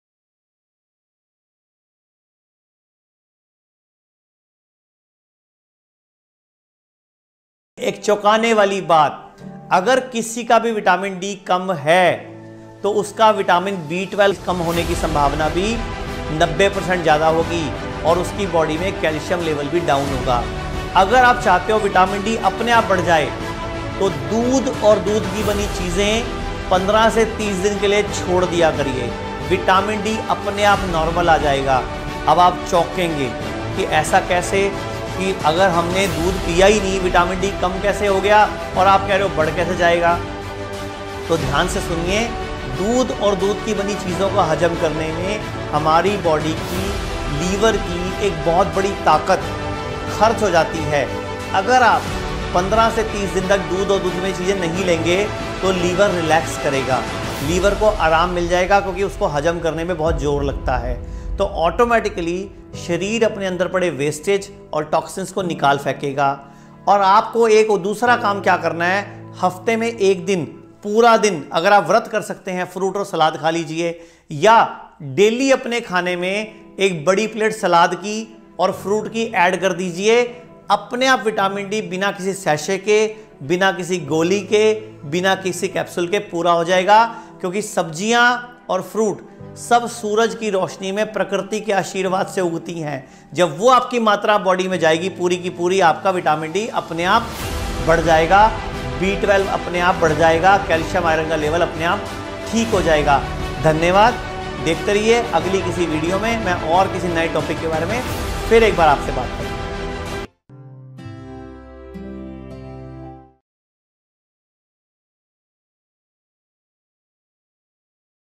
Best Ways To Raise Vitamin-D | How to increase Vitamin B12? | Sources of Vitamins by Guru Manish Ji
Do you have vitamin D deficiency?

There are natural ways to increasing vitamin D levels in your body. And Guru Manish Ji, in this video, explains how to increase vitamin D and B12 naturally.
Along with this, he also gives tips for healthy eating habits.

Follow us for such information.

0:00 What happens when low on vitamin D?
0:22 How quickly can I get my vitamin D levels up?
0:36 Does dairy cause vitamin D deficiency?